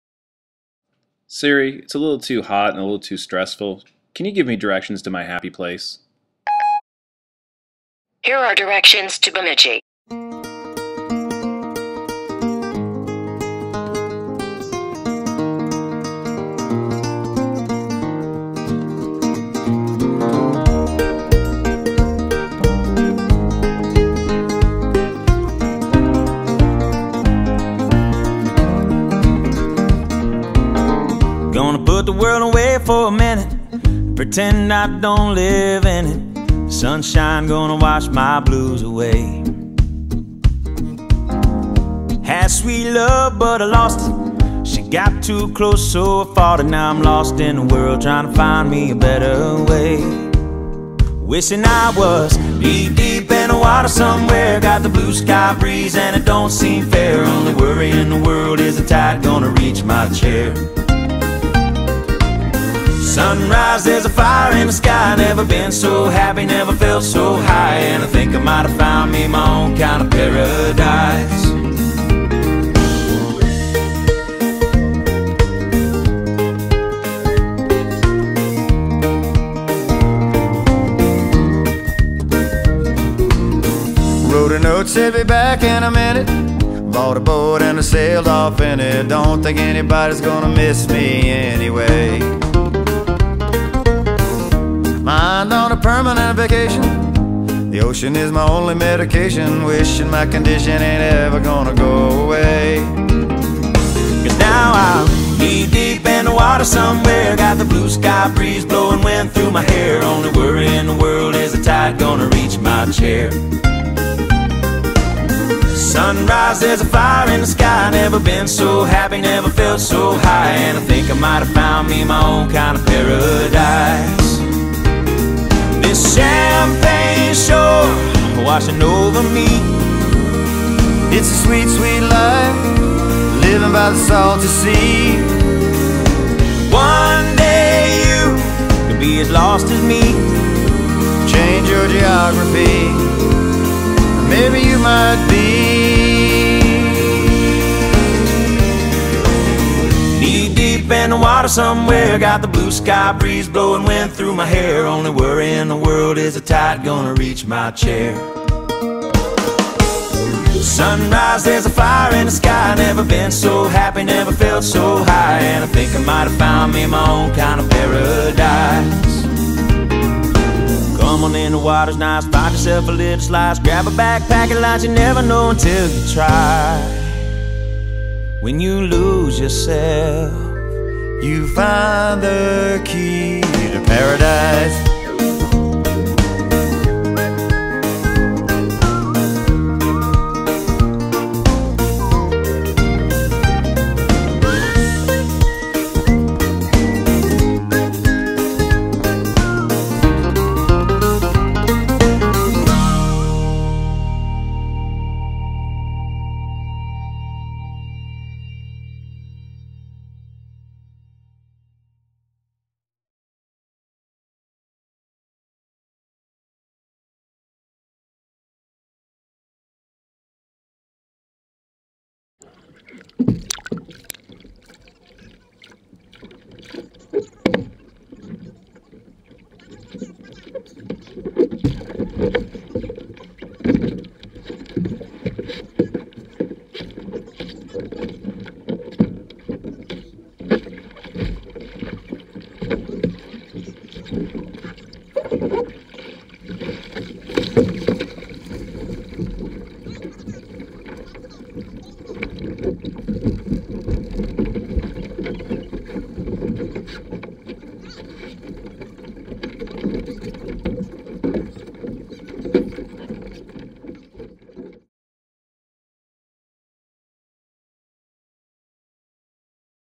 Bemidji....More than just a fun word to say. It's a fun place to stay.
Bemidji, Birthplace of Jane Russell, NFL hall of fame inductee Dave Casper, and Paul Bunyan. It is the first city on the Mississippi and 15 miles from the continental divide. It is also my happy place and a great place to vacation.